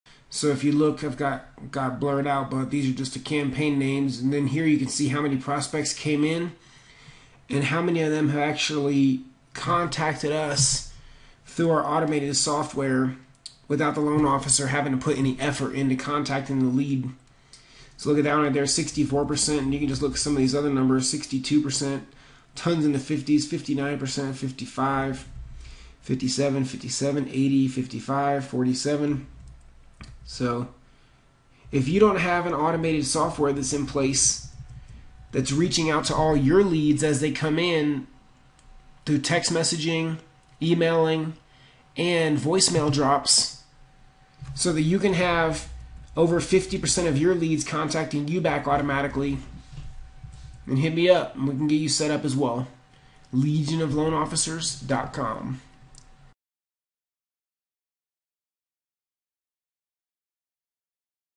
Loan Officer Getting Over 50% Contact Rate From Mortgage Facebook Ads
Check out this quick demo how John in North Carolina is averaging over 50% contact rate with exclusive home buyer and mortgage leads from Facebook ads.

Learn the same ads and process in The Legion of Loan Officers